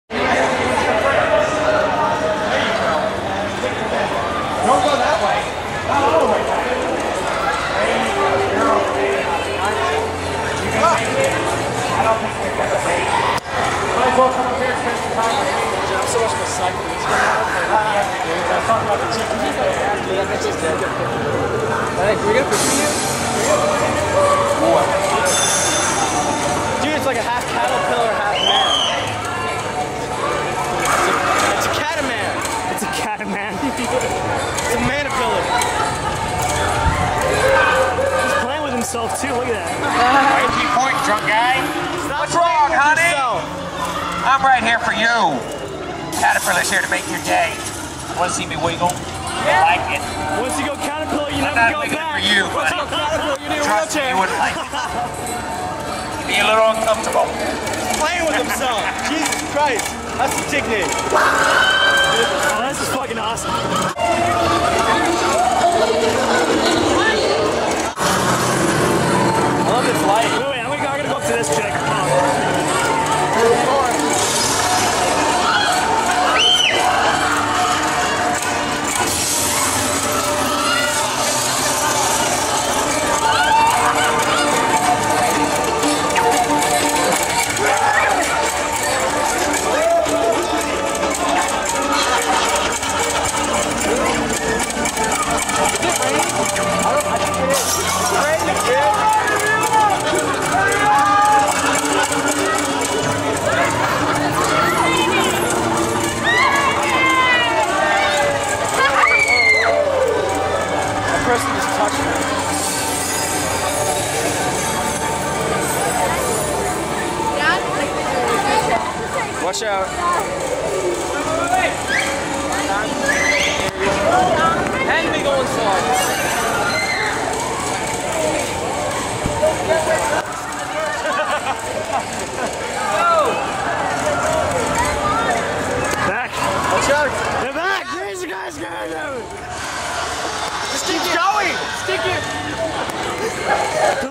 Asylum in Wonderland Walk Through
Walk through of Asylum in Wonderland at Halloween Horror Nights 2008.